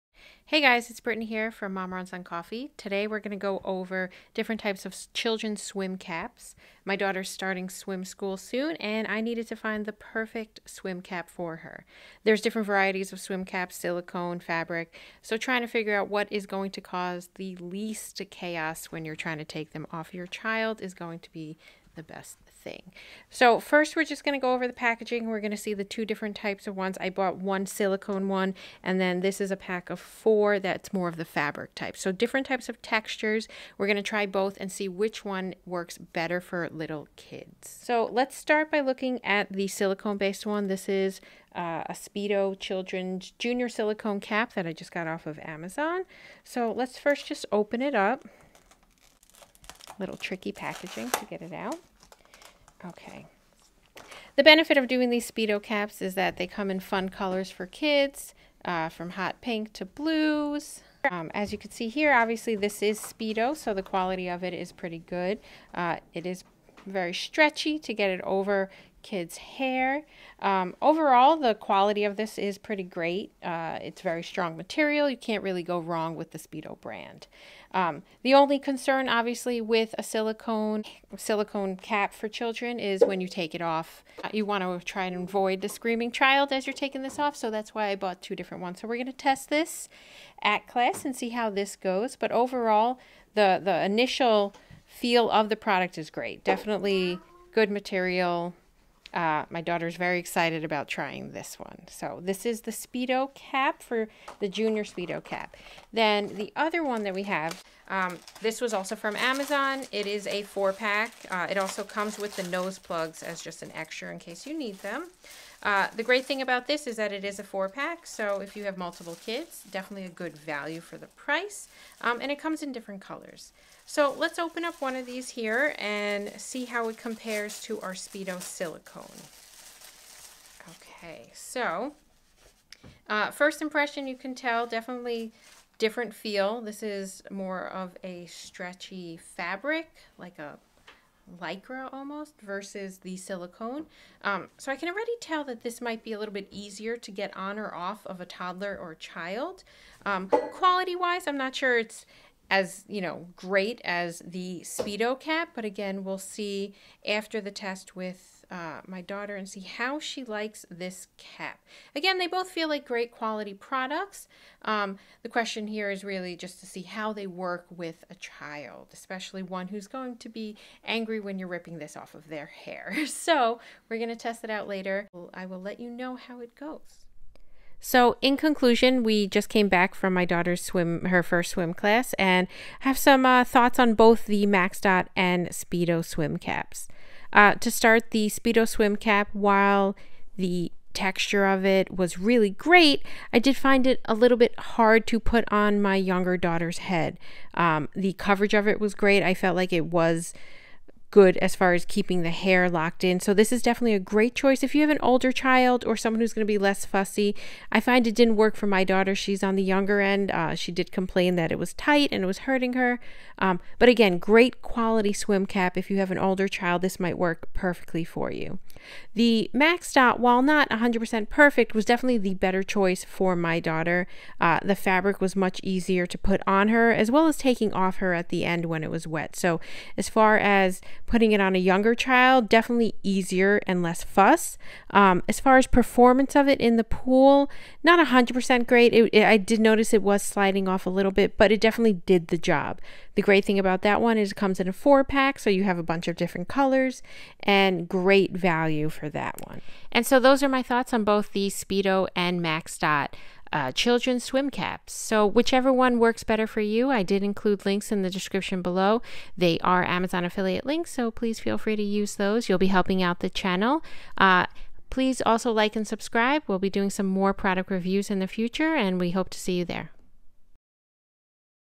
What Children’s Swim Cap to Buy?
In this video, I review two options for Children's swim caps. Silicone swim caps vs. fabric swim caps. Which is the better value for your family when it comes to children's swim caps.

Links to the products reviewed in this video are below.

Speedo Silicone Junior Swim Cap

Maxdot 4 Pieces Swimming Caps